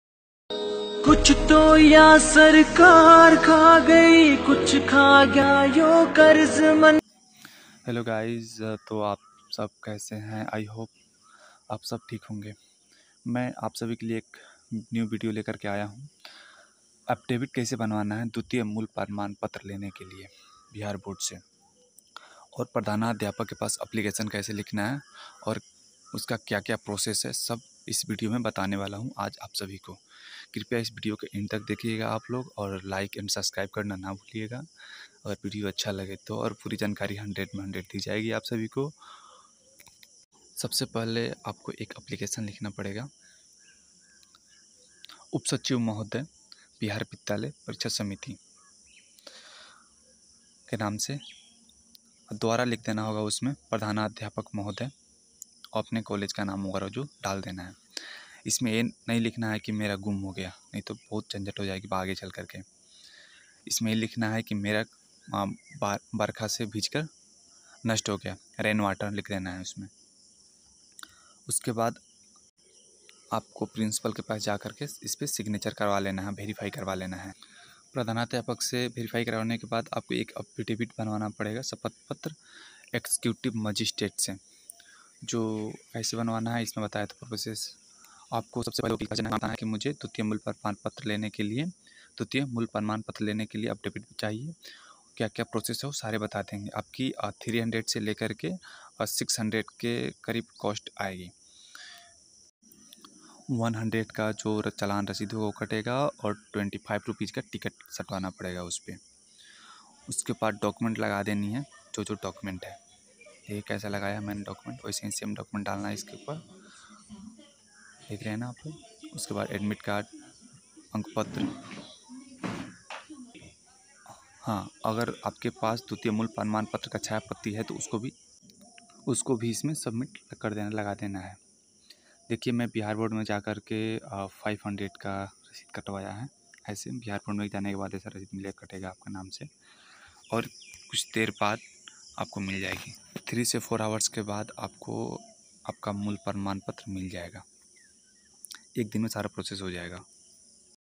dwitiya Mul praman Patra lene ke liye क्या-क्या document chahie aur application kaise likhe
dwitiya Mul praman Patra lene ke liye क्या-क्या document chahie application kaise likhen iske bare mein pura ful video banaya hun video ko dekhiaega aap log aur like and subscribe karna na bhulen thanks 🙏👍 watch me this video